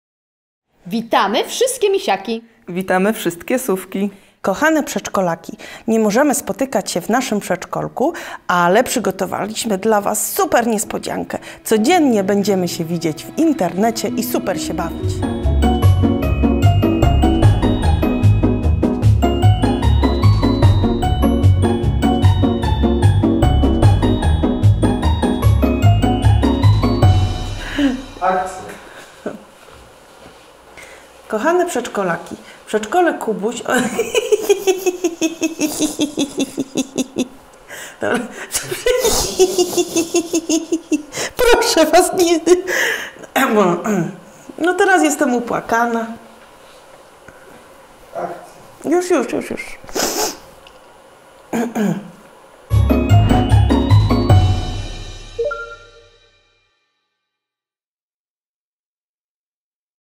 Codzienne zabawy przedszkolne - zapraszamy
Nie poddajemy się! Codziennie walczymy z wirusem zabijając nudę i wywołując uśmiechy dzieci (i nie tylko) 😂